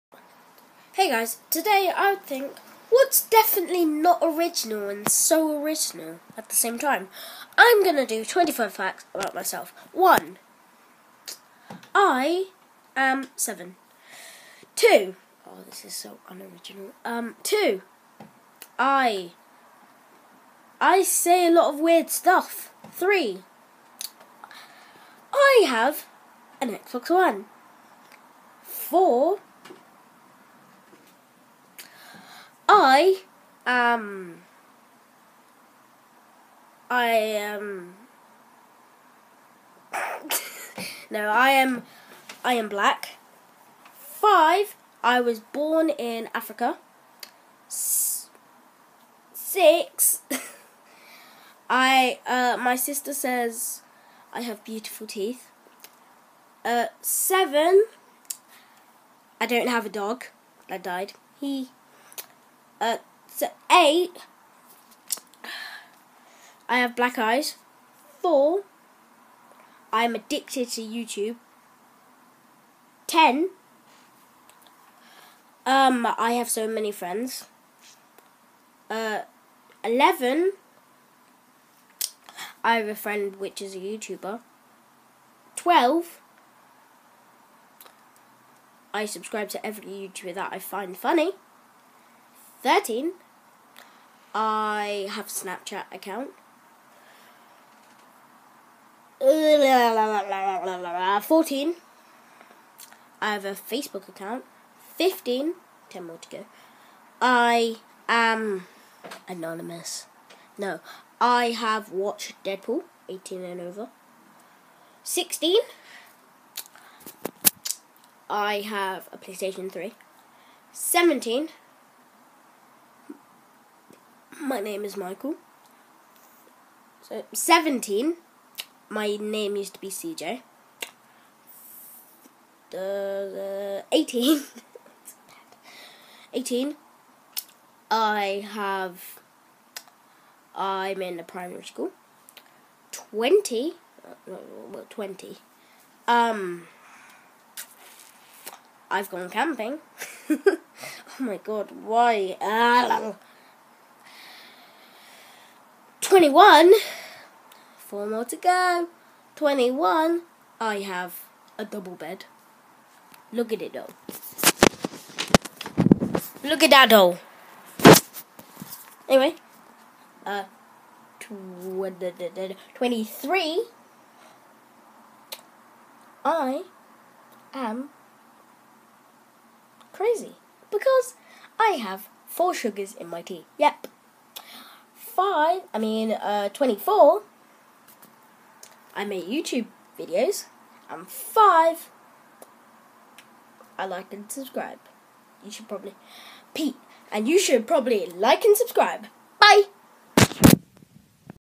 25 Facts about me ( so unoriginal )
I talk about 25 facts about me